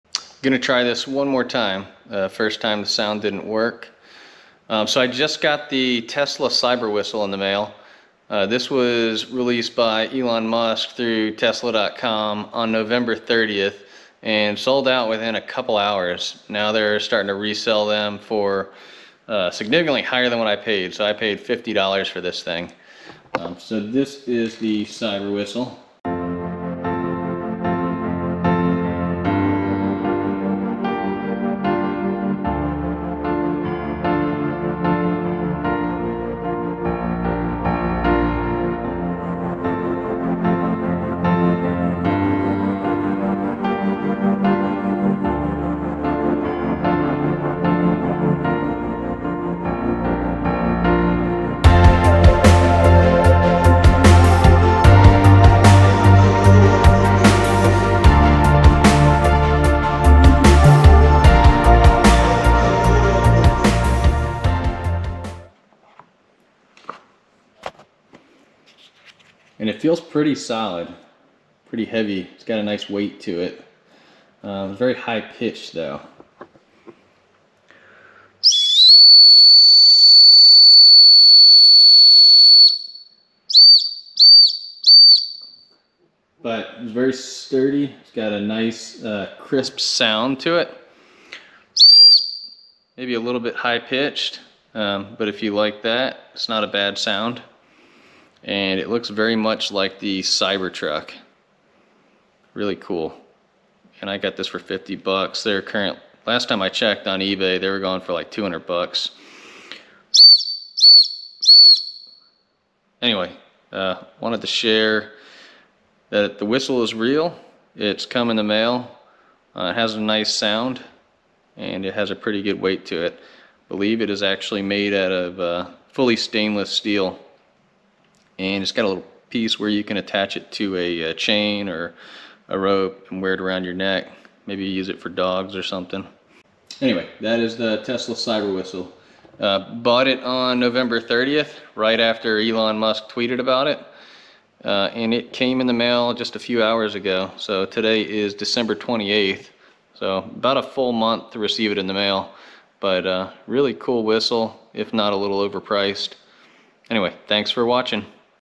Tesla Cyberwhistle Unboxing And First Use
Ordered on November 30th, sold out within 2 hours of going live.  I was lucky enough to catch Elon Musk's tweet about it right when it went live.  In this video I unbox (arrived on December 28th) and test the whistle.